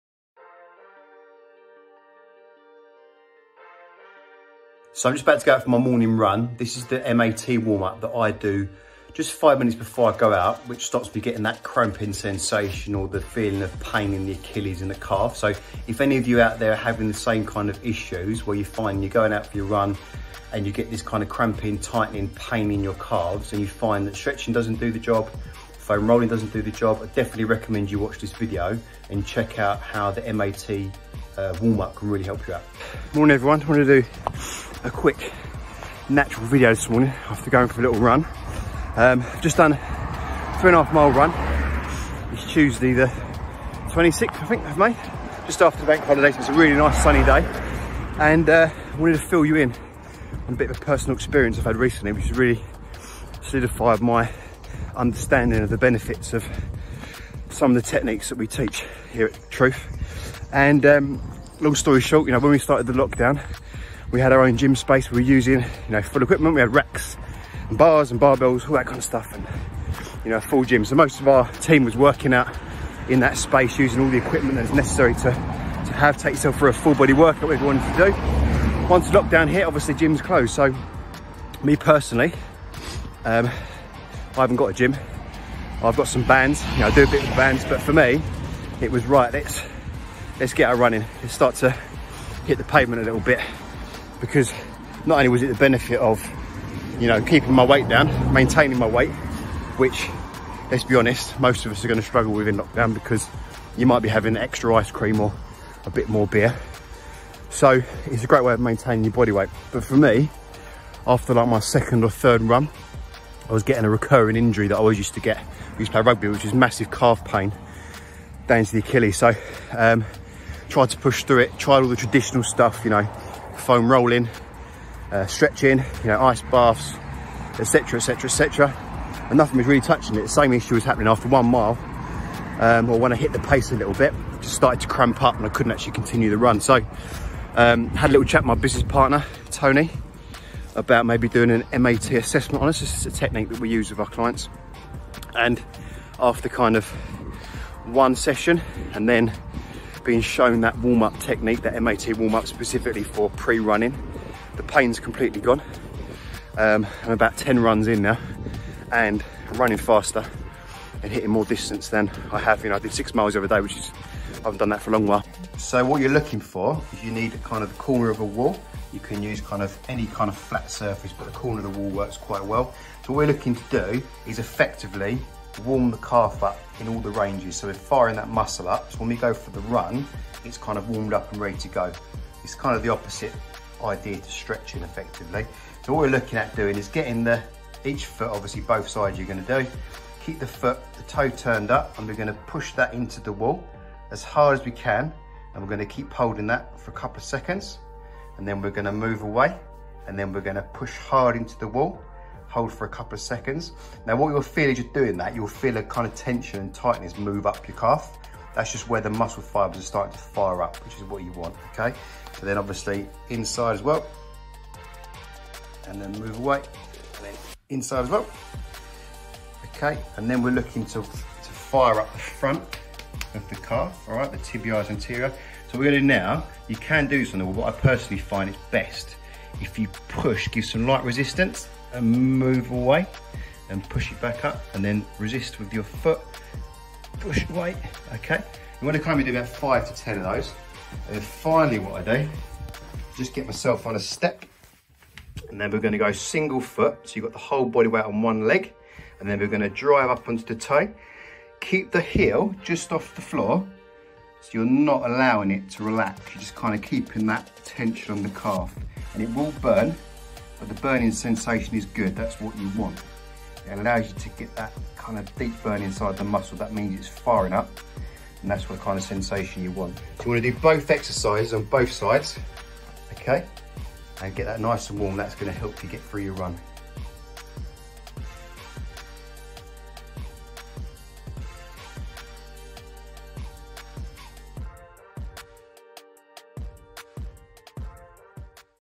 How To Fix Your Calves for Running
If you're about to head out for a bank holiday run, then check out our video, "How to fix your calves for running."

The solution to those tight, painful calves that always stop you from finishing your run…

for your FREE training, Five Steps to Rapid Weight loss